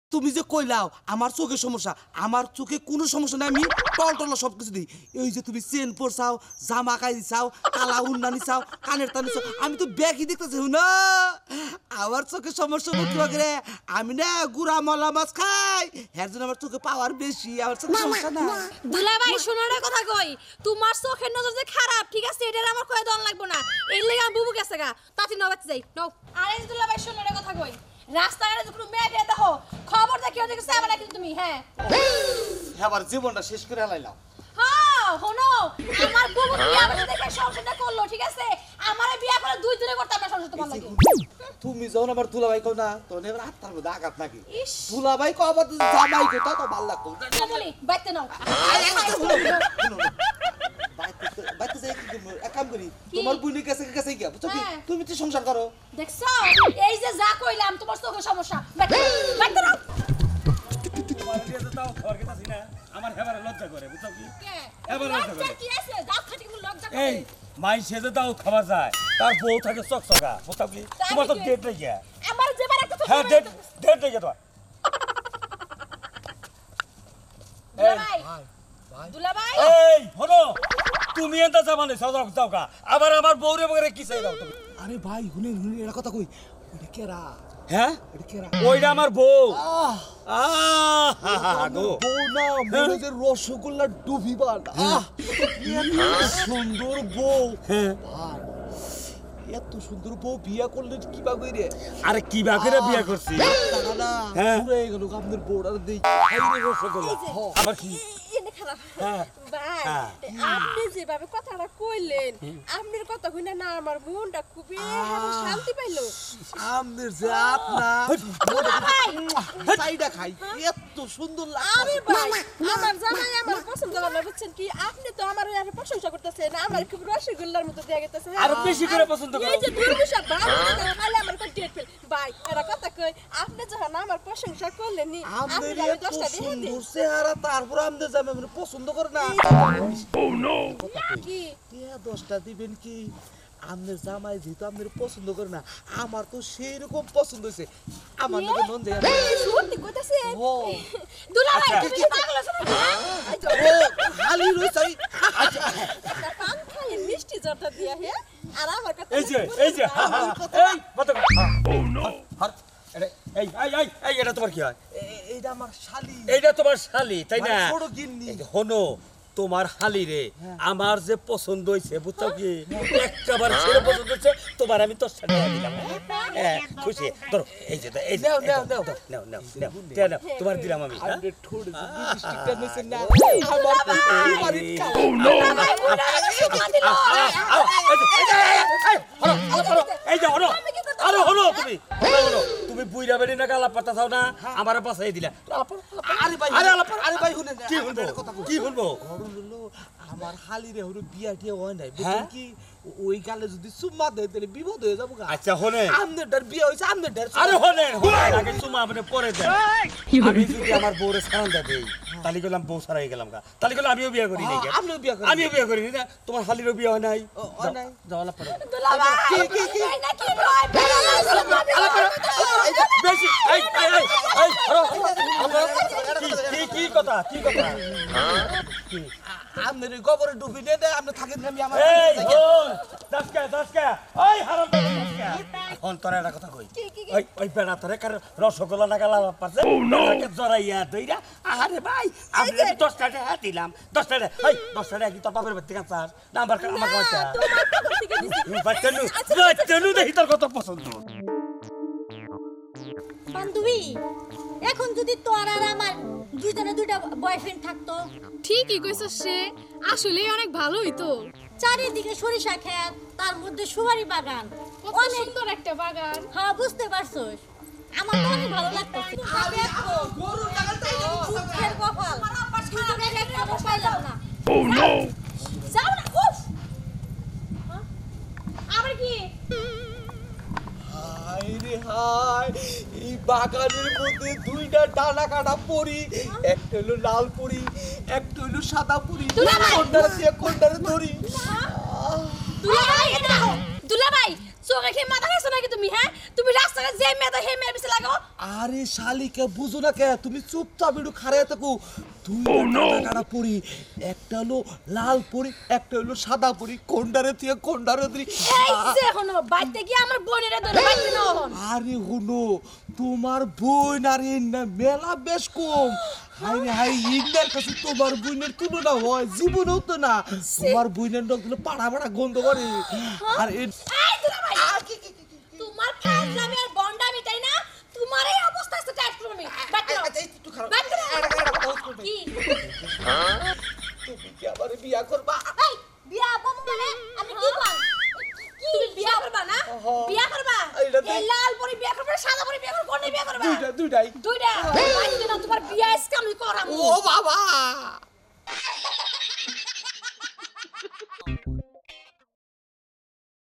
সুন্দরী মেয়ে দেখলেই ভোম্বলের বিয়ে করতে ইচ্ছা করে | রবি চেংগু ভাদাইমা । New Bangla Koutuk 2022
🚫 Anti-Piracy Warning 🚫

This Content is Copyright to Sadia Drama . Legal action will be taken against those who violate the copyright of the following material presented ❗